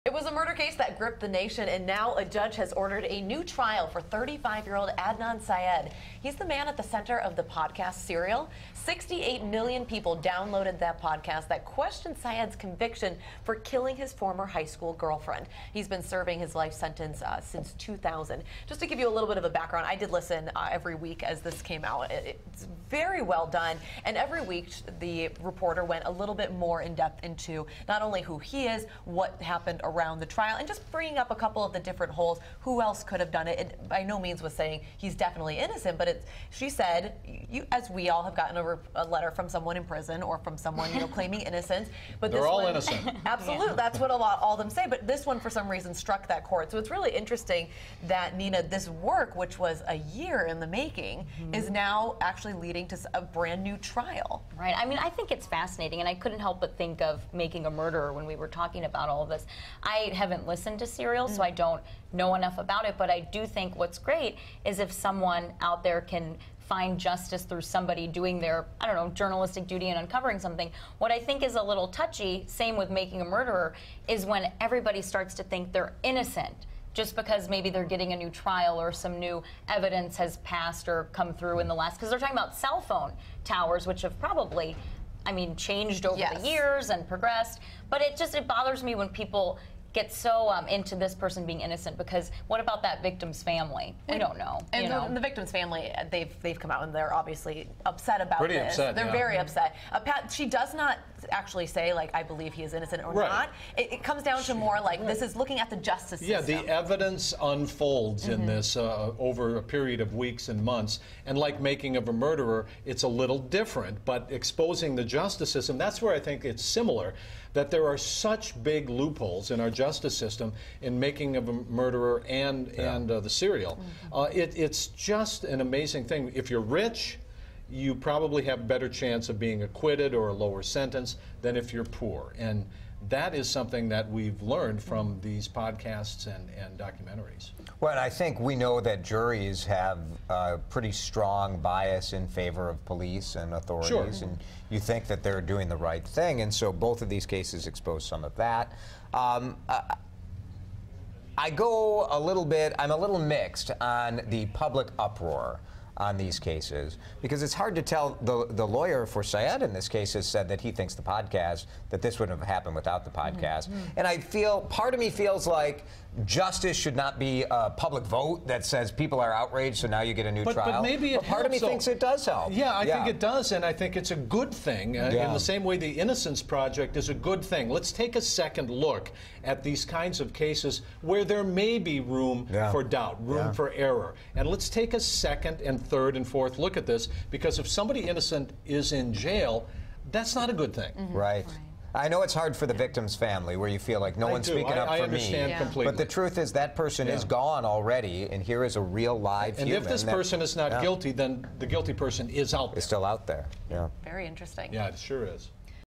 'Serial' Subject To Get New Trial
Adnan Syed, the man at the center of the popular podcast "Serial," is getting a new trial, Jason DeRusha, Kylie Bearse, Nina Moini and Pat Kessler report (3:25). WCCO WCCO Mid-Morning – July 1, 2016